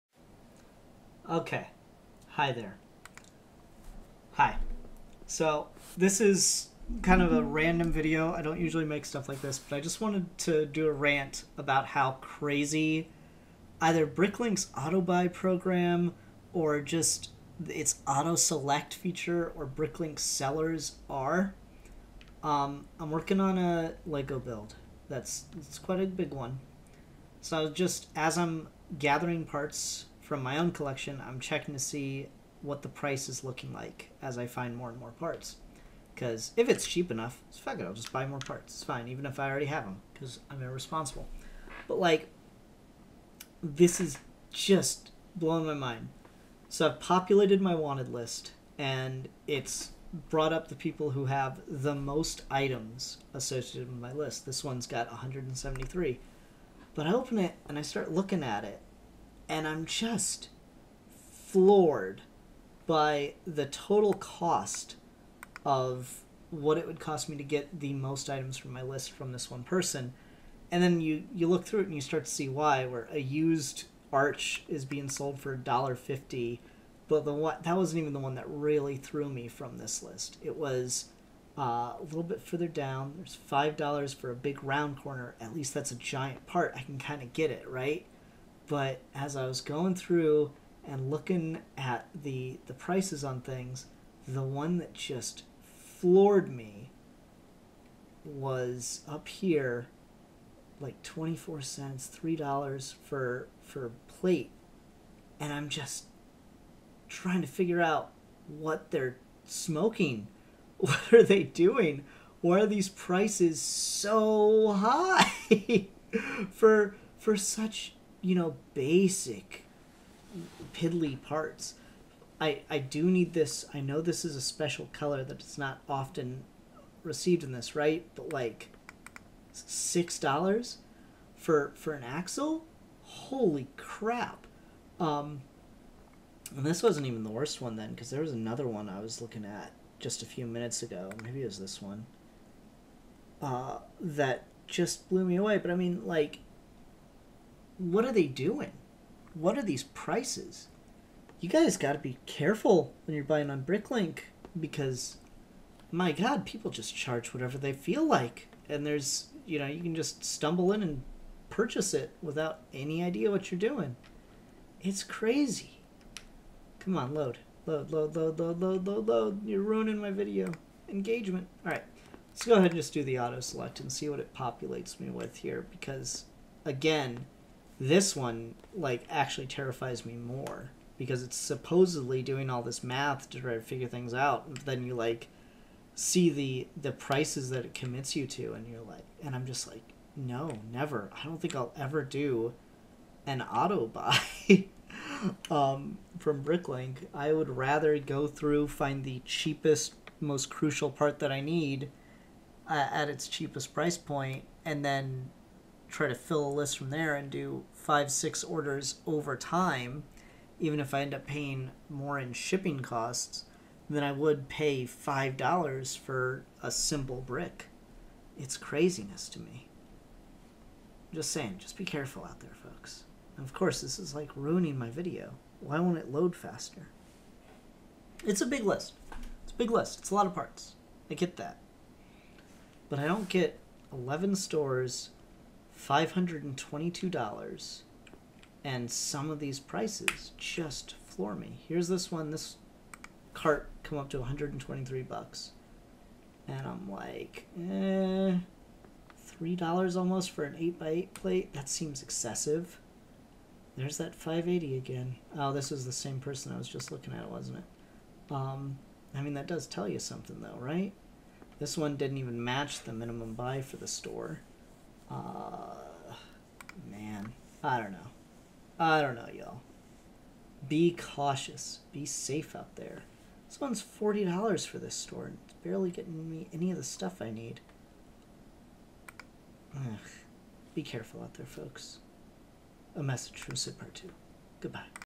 Brick Link Auto Buy Rant
I'm just floored by some of these prices.